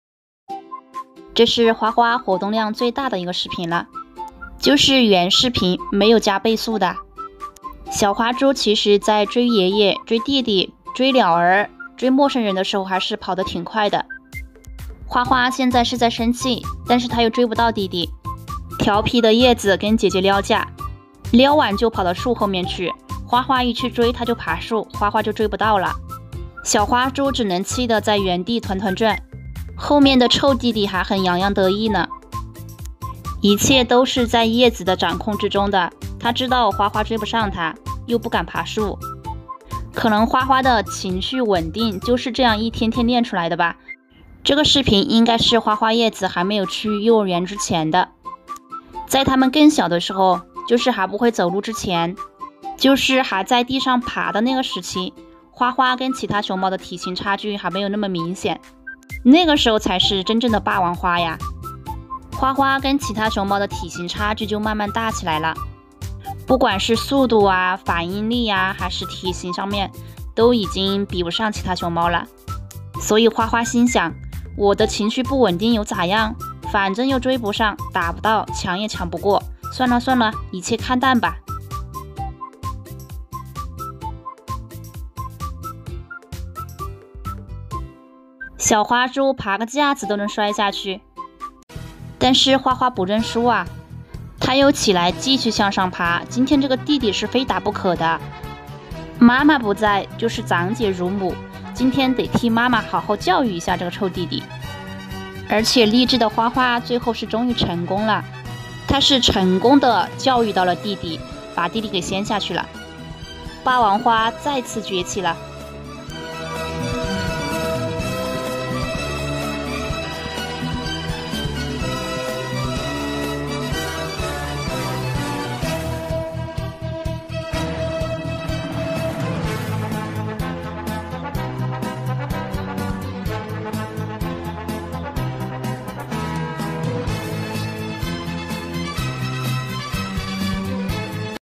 花花运动量最大的视频！叶子撩完架就爬树 花花一直追 气得团团转
大家好我是小伊闲谈
一名熊猫狂热粉
"我们明确遵循了保护动物权益的原则，以确保视频中没有发生任何形式的动物伤害。"